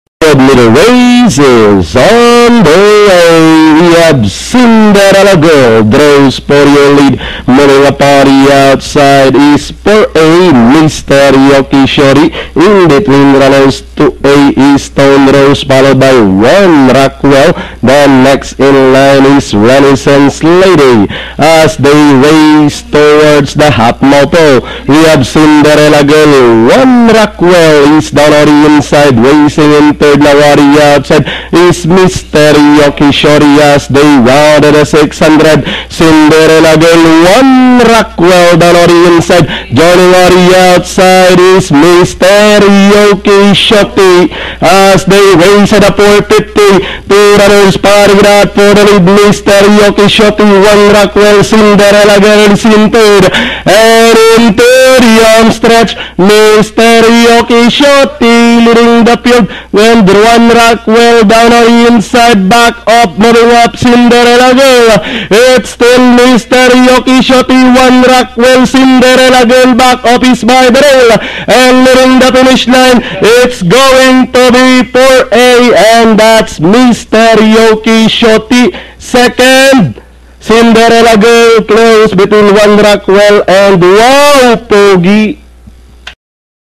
PRCI-031814-R08
RACE 8 Results 4/5/1/6    Favorite 4
WINNING HORSE -- MISTER YOKISHOTI